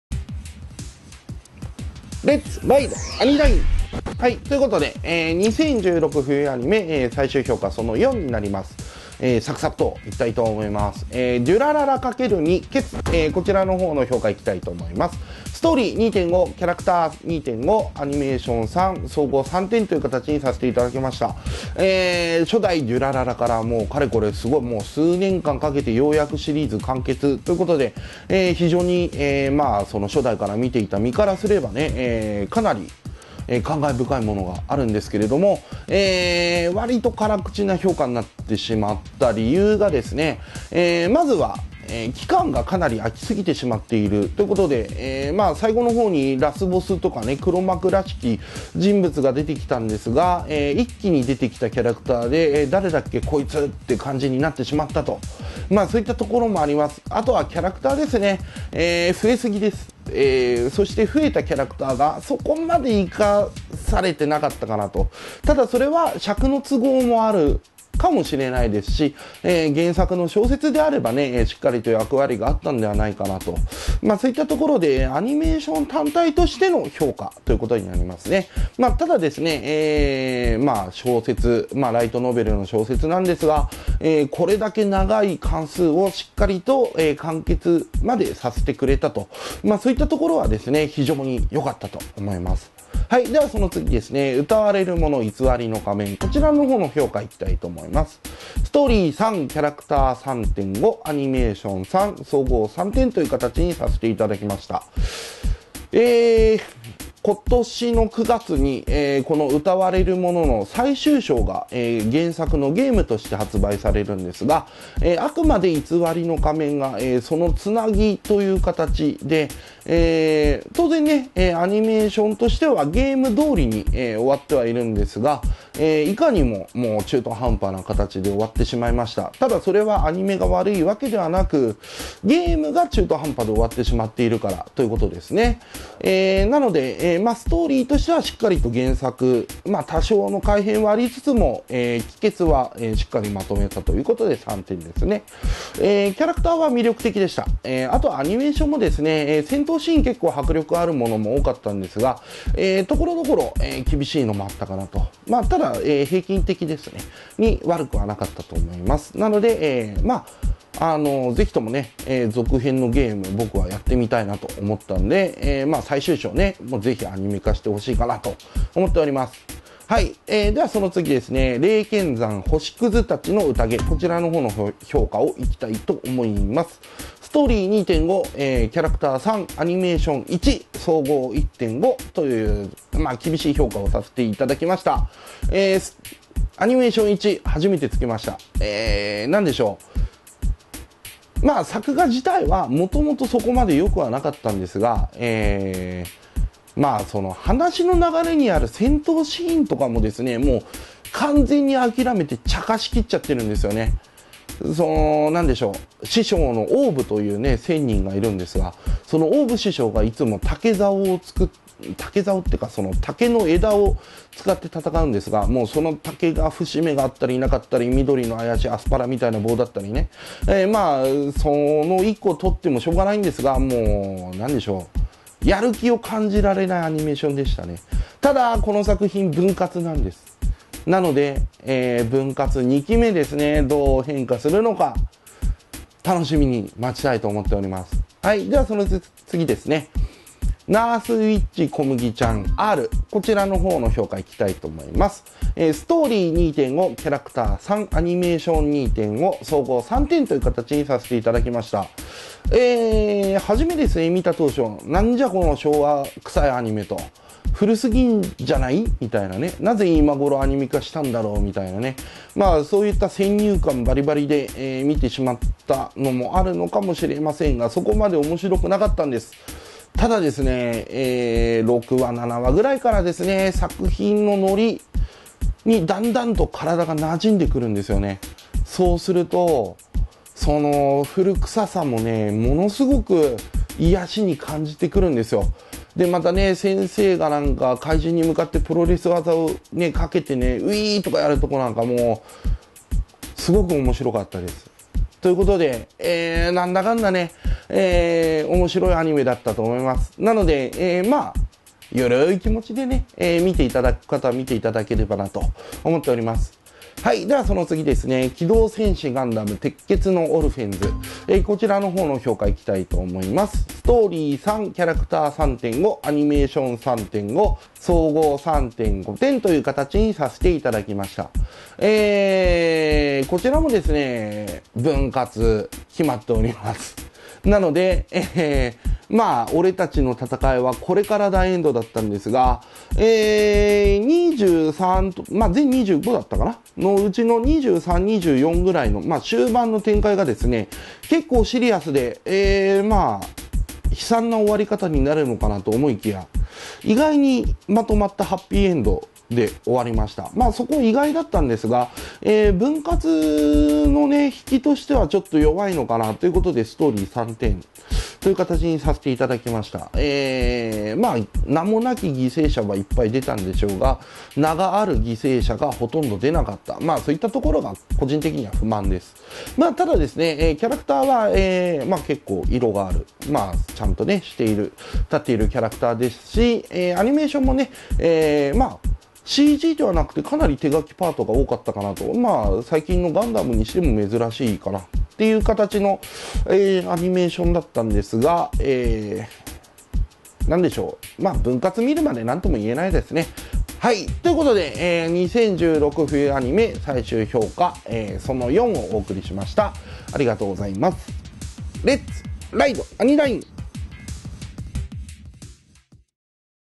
2016冬アニメ最終評価・感想【辛口】④/6
アニメ紹介番組「INADANIME.ch」#32
今回は「2016冬アニメ最終評価・感想その４」です。INADA関連サイト

18 デュラララ！！×2　結
19 うたわれるもの　偽りの仮面
20 霊剣山　星屑たちの宴
21 ナースウィッチ小麦ちゃんR
22 機動戦士ガンダム　鉄血のオルフェンズ